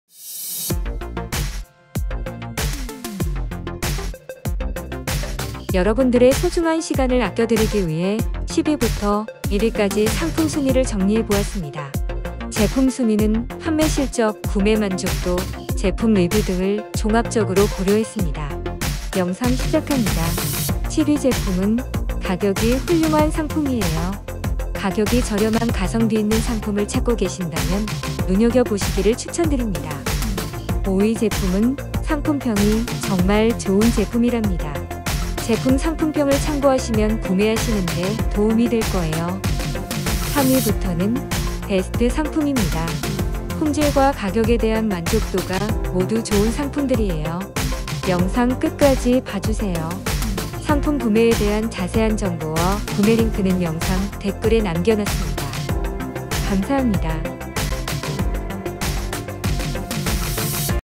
모블랩스거치대 Top10! 가격, 평점, 후기! 놓치지 마세요!
❤️❤️❤️ 요즘 인기있는 모블랩스거치대 구매 가격 평점 리뷰 정리해 봤어요! ❤️❤️❤️

[00:06]
10위. 특대형 삼탠바이미 거치대 55인치 65인치 TV스탠드 삼성 [279,000원]

[00:11]
9위. 77인치 TV스탠드 65인치 75인치 티비거치대 이동식 삼성 [199,000원]

[00:16]
8위. Montheria 티비거치대 TV스탠드거치대 티비스탠드 B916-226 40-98인치 브라운 [163,000원]

[00:21]
7위. 이동식 삼탠바이미 TV거치대 49인치 LG 삼성 호환 [139,000원]

[00:26]
6위. UNHO 특대형 TV스탠드 높이조절 65-100인치 거치대 우드+철제 [235,000원]

[00:31]
5위. 모블랩스 삼탠바이미 M5 M7 M8 티비거치대 삼텐바이미 [125,000원]

[00:36]
4위. 모블랩스 이동식 티비거치대 삼텐바이미 삼탠바이미 32인치 무타공 [121,000원]

[00:41]
3위. 모블랩스 삼성 M5 M7 리모콘 거치대 포함 [120,000원]

[00:46]
2위. 모블랩스 삼성 M5 M7 리모콘 거치대 포함 [114,000원]

[00:51]
1위. 모블랩스 삼탠바이미 티비거치대 삼성 M5 M7 LG [129,000원]

이 포스팅은 쿠팡파트너스 활동의 일환으로, 이에 따른 일정액의 수수료를 제공받습니다.